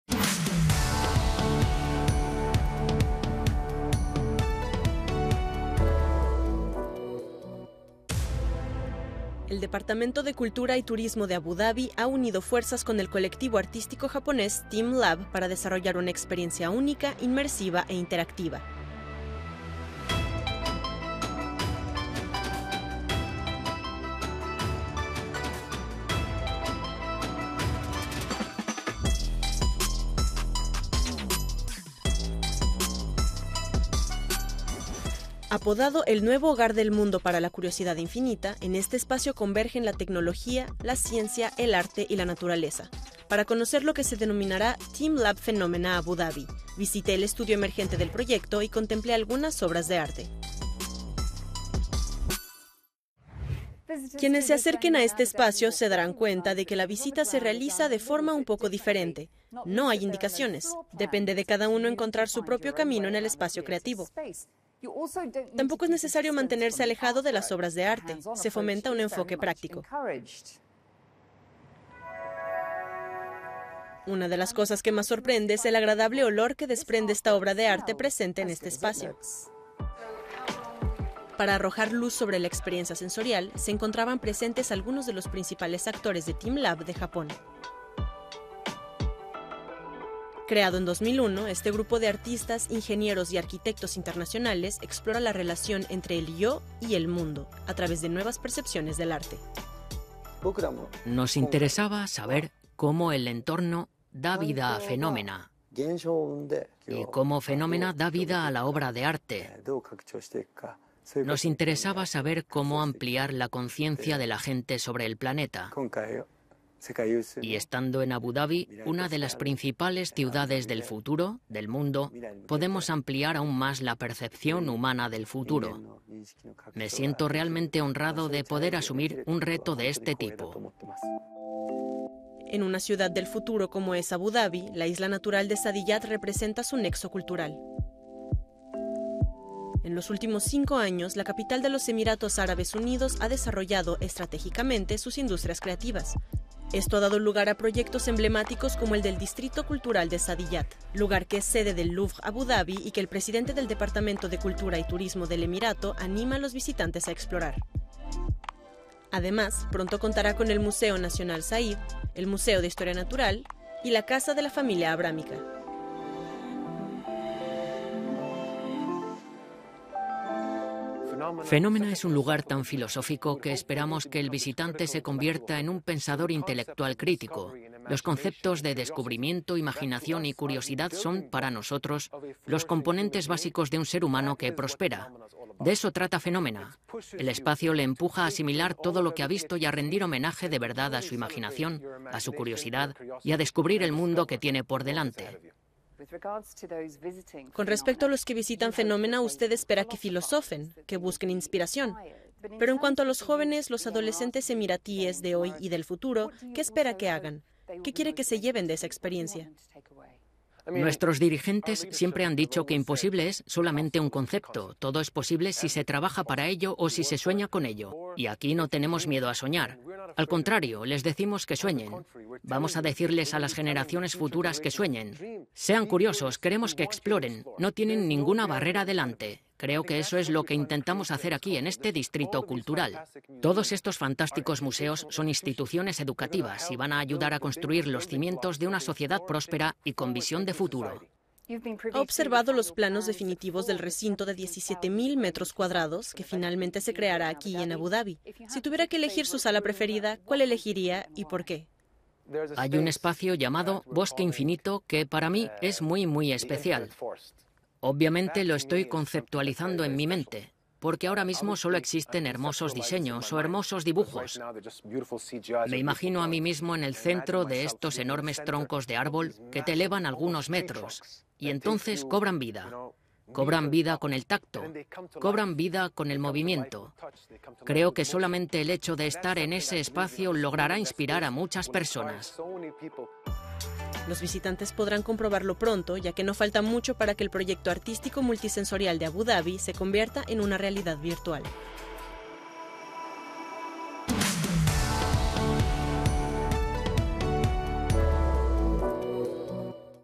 TeamLab Phenomena Abu Dhabi o el 'nuevo hogar del mundo para la curiosidad infinita'
Apodado ‘El nuevo hogar del mundo para la curiosidad infinita’, en este espacio convergen la tecnología, la ciencia, el arte y la naturaleza. Para conocer lo que se denominará ‘teamLab Phenomena Abu Dhabi’, la periodista Rebecca McLaughlin-Eastham, visitó el estudio emergente del proyecto.